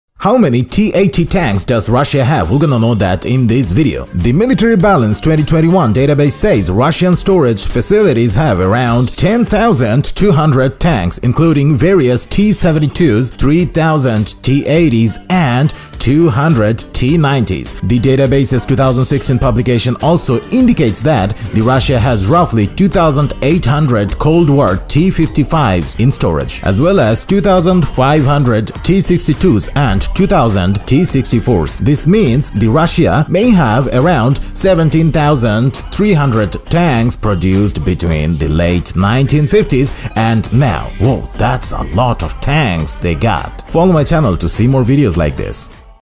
T-80 tanks of Russia
The Military Balance 2021 database says Russian storage facilities have around 10,200 tanks, including various T-72s, 3,000 T-80s, and 200 T-90s.
The database’s 2016 publication also indicates that Russia has roughly 2,800 Cold War legacy T-55s (the first tank type to feature a nuclear warfare protection system in the 1950s) in storage, as well as 2,500 T-62s and 2,000 T-64s.
This means that Russia may have around 17,300 tanks produced between the late 1950s and now.